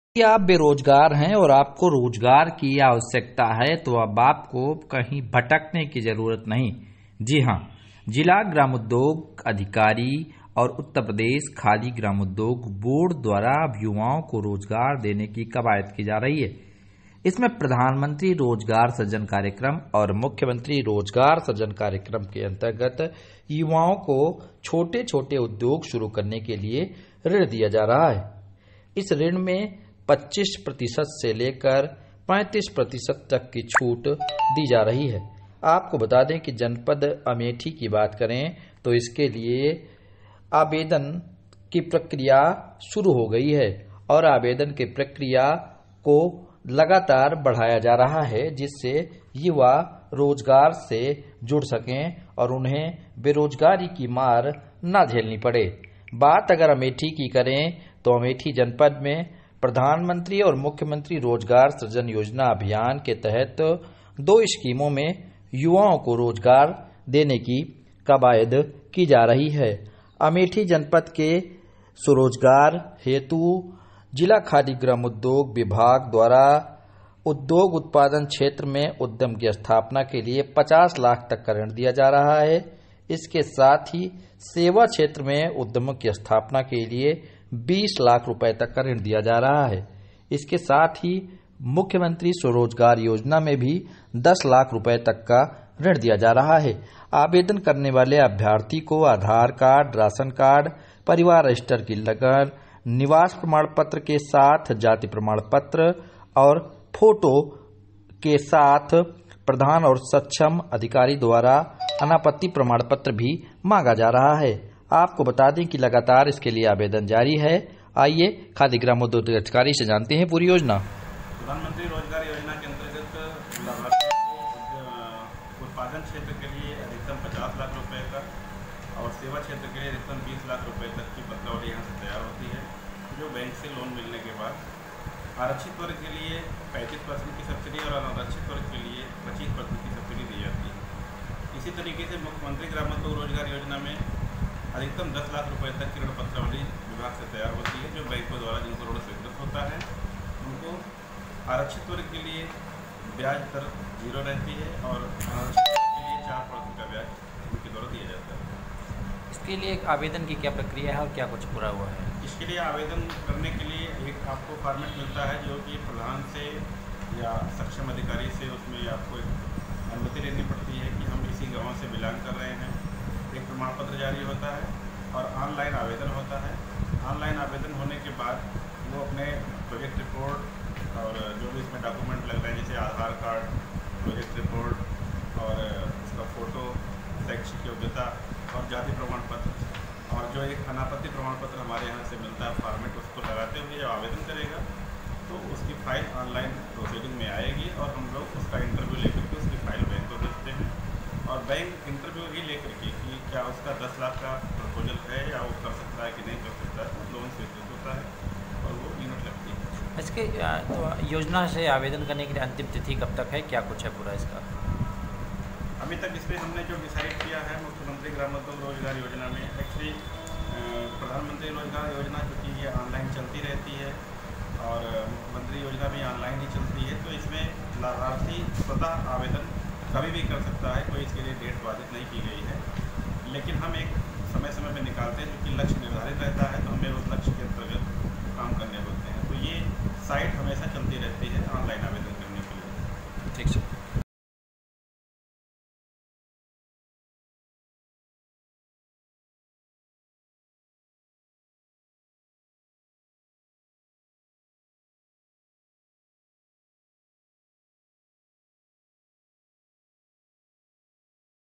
अमेठी का खादी ग्रामउद्योग विभाग दे रहा 50 लाख तक का लोन, ऐसे करें आवेदन
जिला खादी ग्रामोद्योग अधिकारी महेंद्र कुमार मिश्र ने बताया कि युवाओं को रोजगार अभियान से जुड़ने के लिए इस पहल को किया जा रहा है. ताकि युवा रोजगार से जुड़ सकें और उनकी बेरोजगारी की समस्या खत्म हो सके.